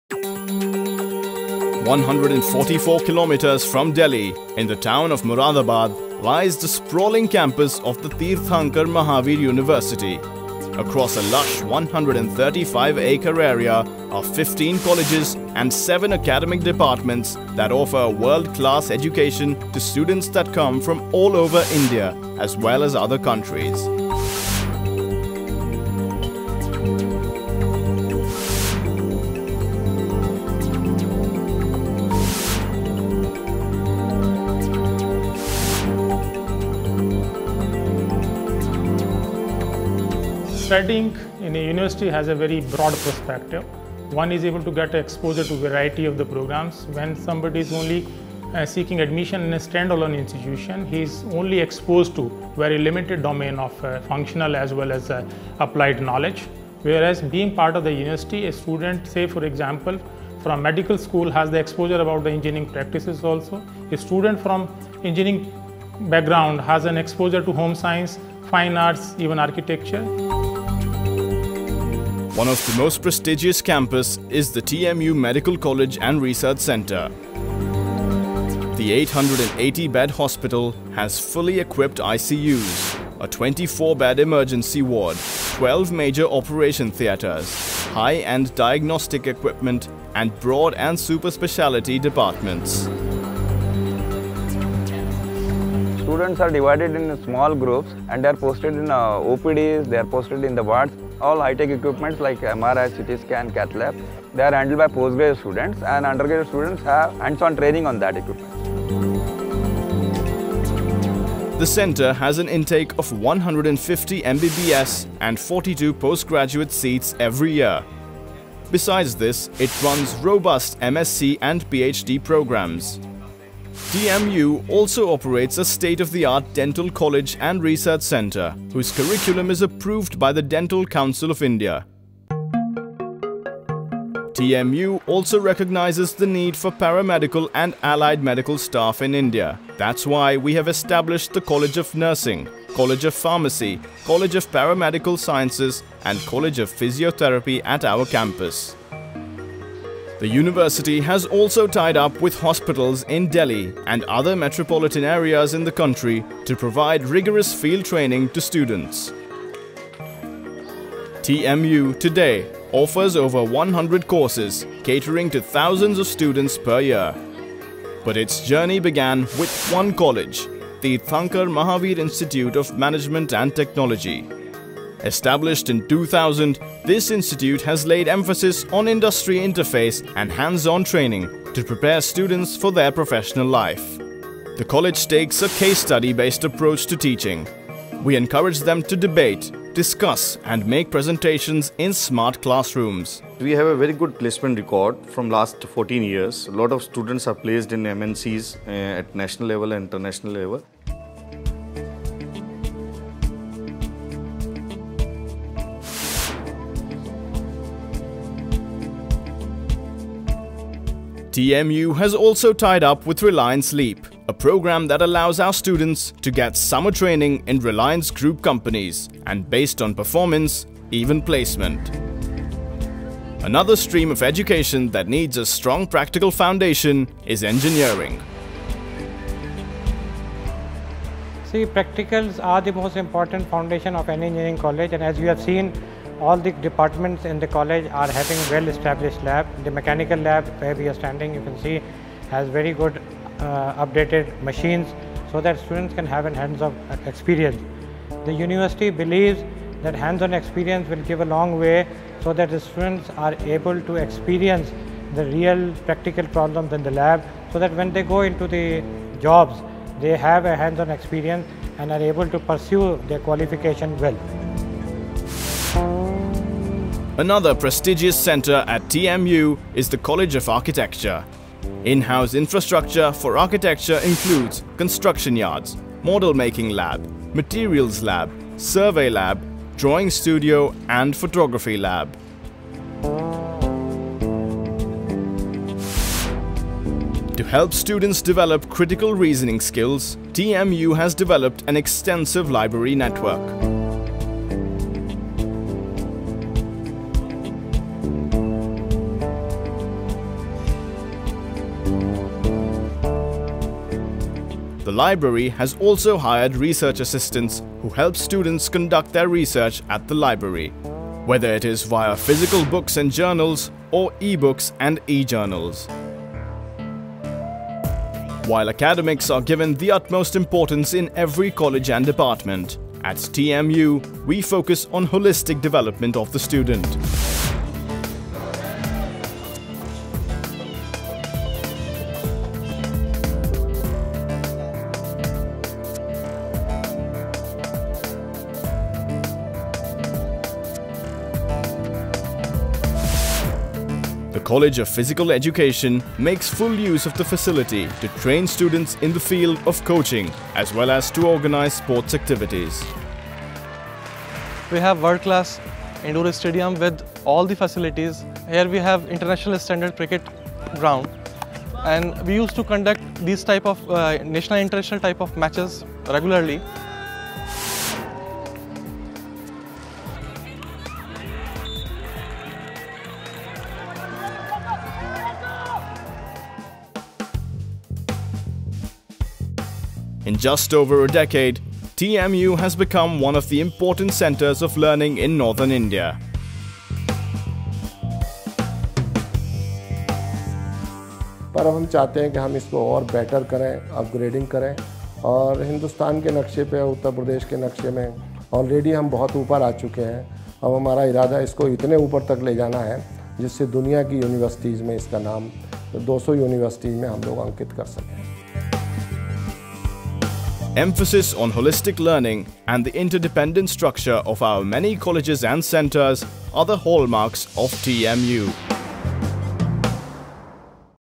TMU Moradabad || Best Private University in UP || Campus Tour
One of the Best Private Universities in UP, Teerthanker Mahaveer University (TMU) Moradabad with NAAC A accreditation and 12 (B) status, provides a cohesive, innovative, and skilled environment for students to grow under the guidance of experienced academicians.

At present, we are offering 120+ career-oriented UG, PG, and Doctoral programs with state-of-the-art infrastructural facilities including labs, an academic approach according to NEP 2020, industry-ready and extensive placement support for the students. The University comprises 12 remarkable colleges. The elite facilities that the wide campus offers include hostels, mess, auditorium, multi-specialty hospital, gym, Bikano restaurant, indoor stadium, 24x7 security, PNB Bank inside the campus premise, and eateries offering multiple cuisines to suit the diverse culture the students belong to.

With
●     100% Placement Assistance
●     International Internship Opportunity
●     Live Interaction with industry stalwarts

And more facilities, making TMU the best private University in UP. Come be a part of the TMU family! to get more information about admission, enrollment, and programme structure.